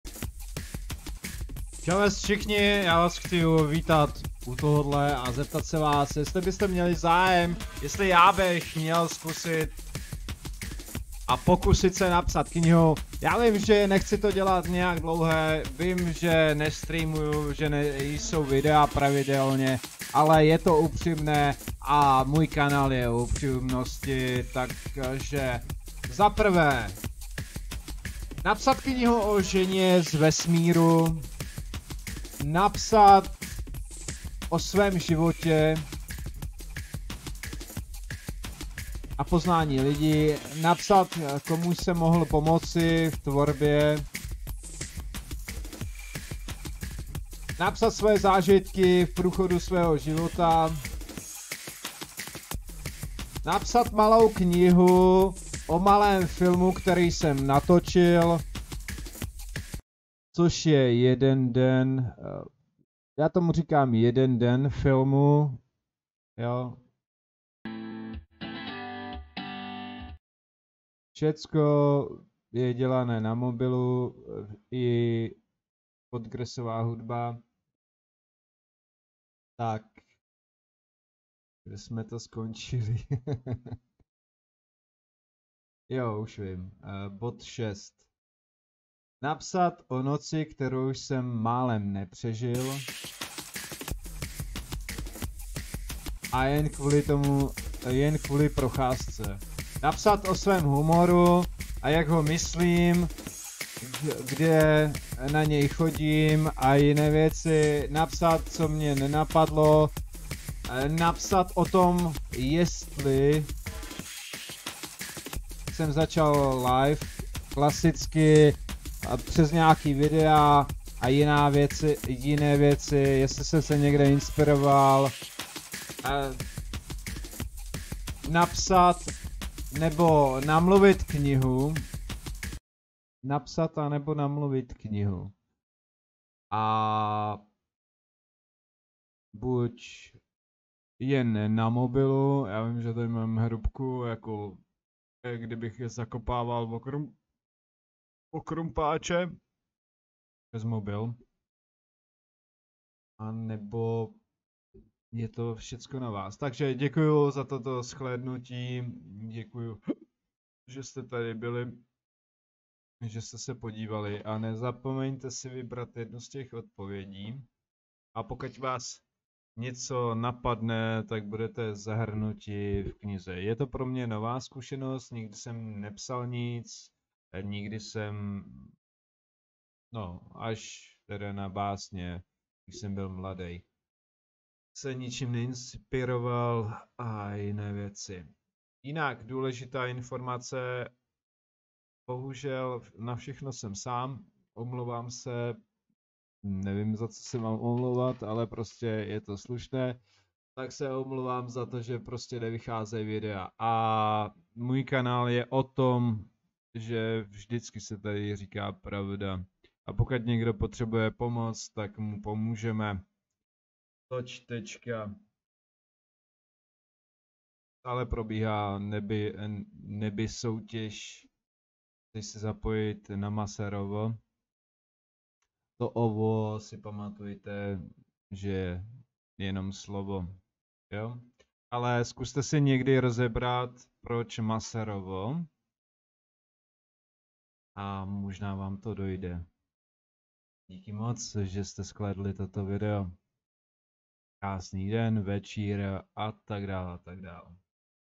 MOJE KNIHA? - jit do neznama !
*** 💰 Latest Tippers:  💰 ***
Když se ti video zamlouvá nezapomeň na komentář, Palec a  sdílení.
 Chtěl bych to jen zkusit ,třeba se to podaří. a ty můžeš vybrat očem to bude 😉
Nevím jak to napsat ale nějak to napišu, film co jsem natočil byl velmi těžky, a napsat vice než jeden řadek , třeba i mluvene knihy je nejspíše větší vyzva v jednom člověku, než dokončeny film ( ktery jsem chtěl nekokončit, a přesto ho dokončil i když ma 12 minut a nespočet dat i chyb, jako neprofesionalni zařizeni, rospočet Skoro 0 a utrap spojenych s nim ) a však dokončil jsem ho! ted mam strach skusit napsat knihu skoro ve stejnych podminkach. A vy se mužete trošku podilet na tomto obsahu přes odkaz a video ktere vzniklo pro tento učel!
► Shop
Pořádný Nářez , Pořádný Hadry!
Můj vzhled ( Disign ) za který se nemusím stydět .

MASEROVO MOJE KNIHA?Jit doNeznama!